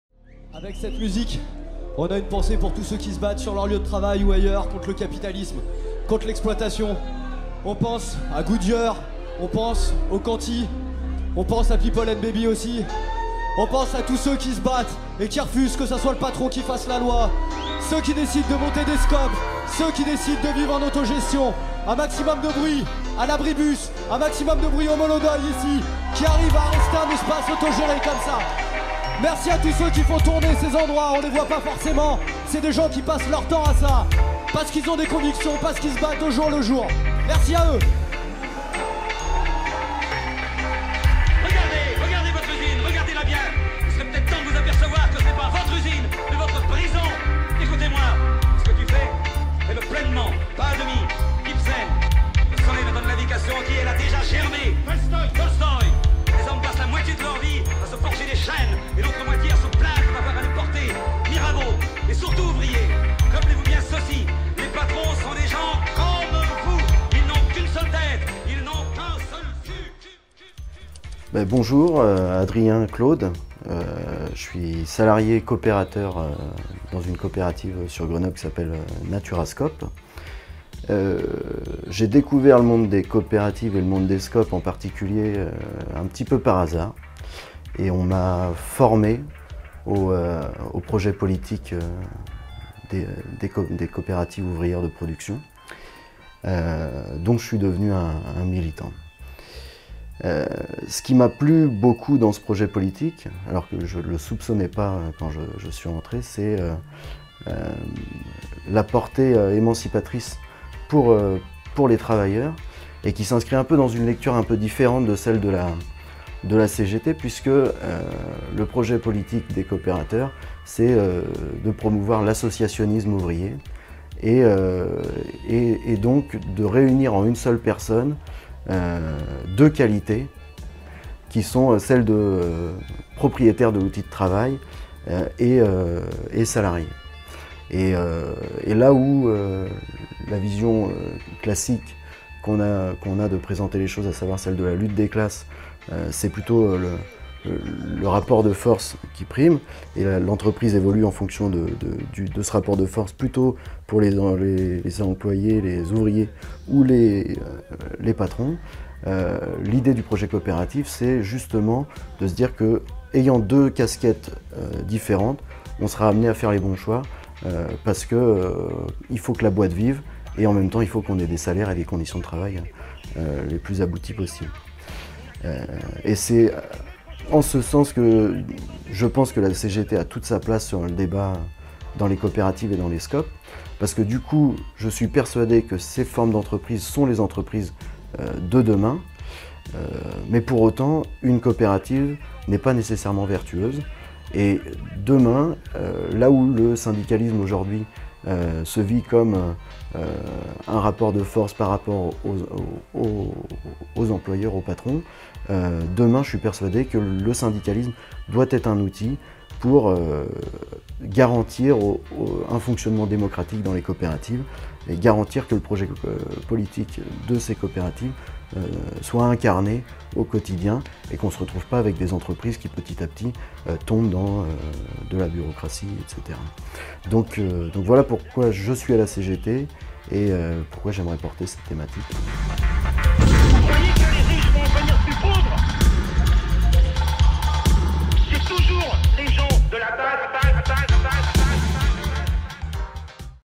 Scop - Clip 3
Retrouvez l'interview d'Adrien sur le thème des Scop